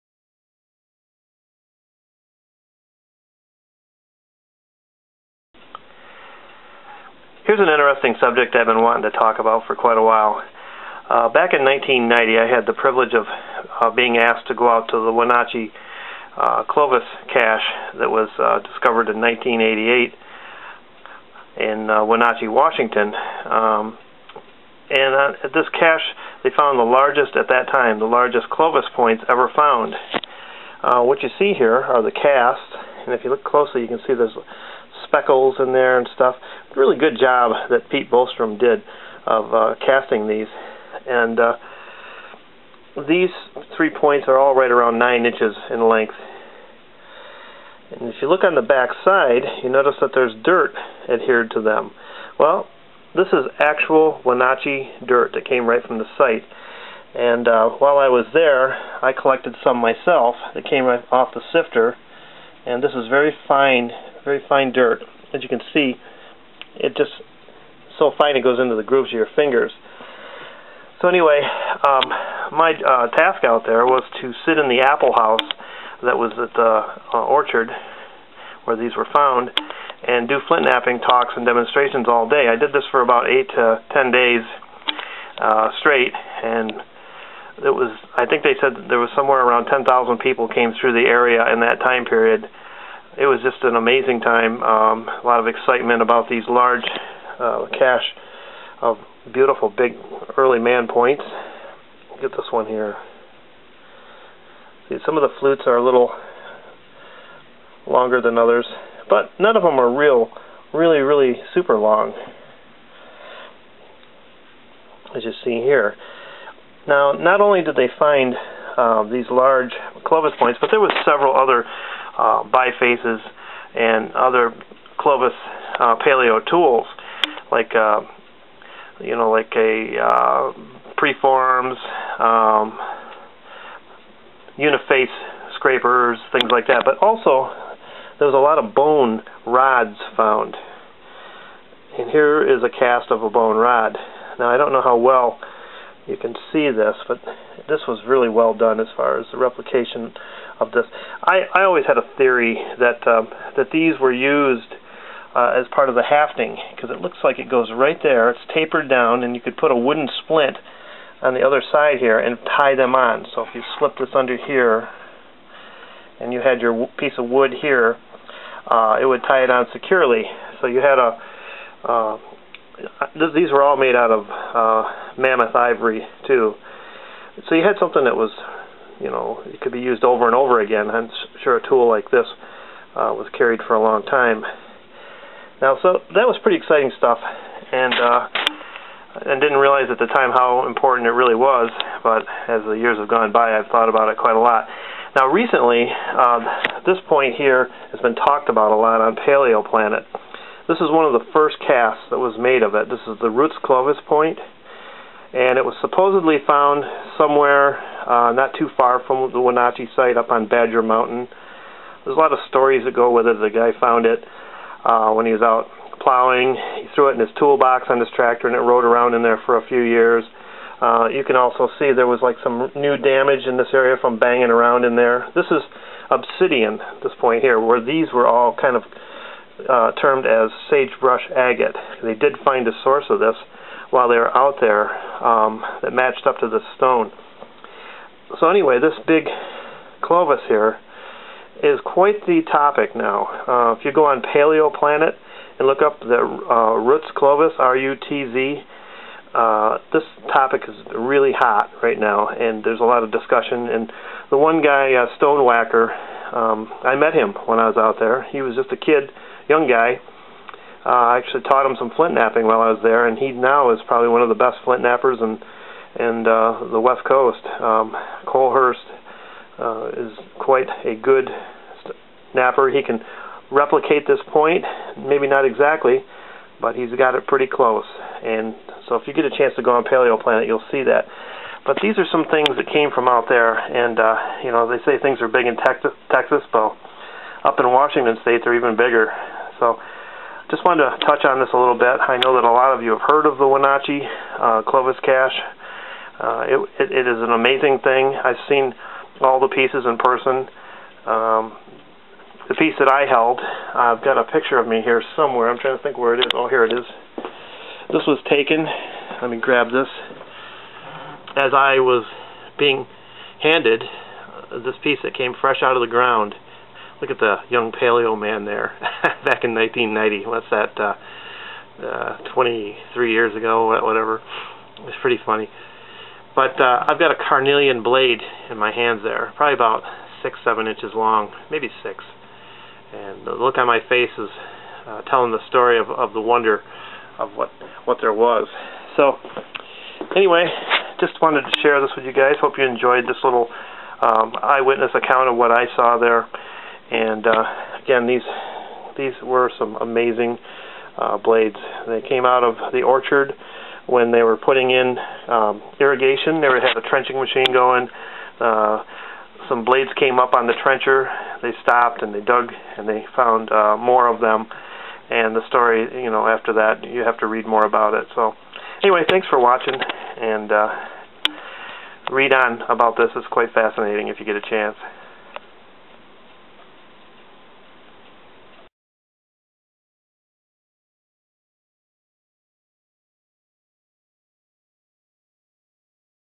Paleoman52 at the Wenatchee Clovis Site
Back in Late October of 1990 I was invited by the Buffalo Museum of Science to fly out to the Wenatchee Clovis site that was being excavated by Buffalo Museum Personnel. My job was to sit in the apple house that was in the orchard where the cache was discovered and give flintknapping talks and demonstrations to the tour groups that came through the site all day for about 8 - 10 days. This video shows the casts of the largest 3 Agate points, a bone rod, and some soil from the site. I also show a cast of the Rutz Clovis point that was found nearby on Badger Mountain. These points were featured in the 1988 October edition of National Geographic Magazine.